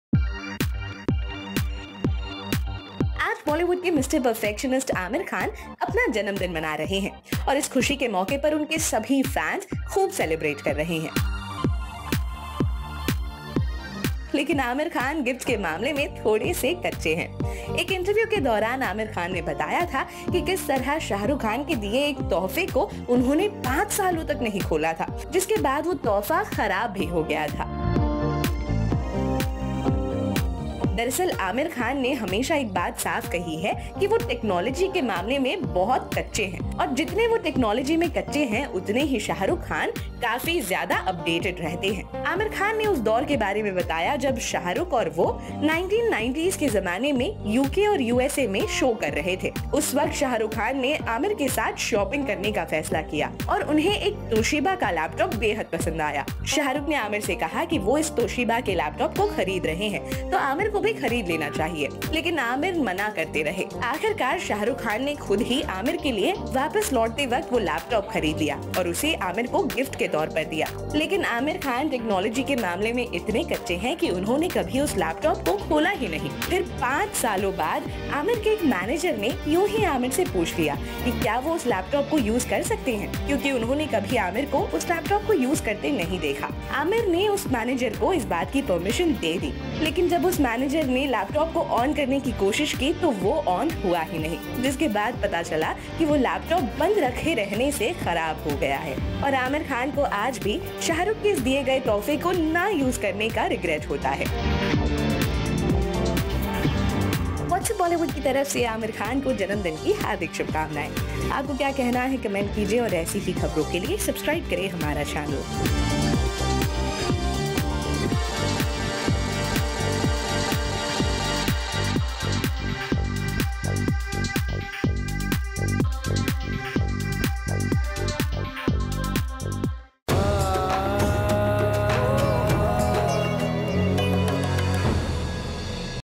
जब Aamir ने 5 सालों तक नही खोला SRK का तोहफा!
जानिए आखिर ऐसी कौनसी वजह थी की Aamir ने 5 सालों तक नही खोला SRK का तोहफा! ...Follow us for more Bollywood Updates!!!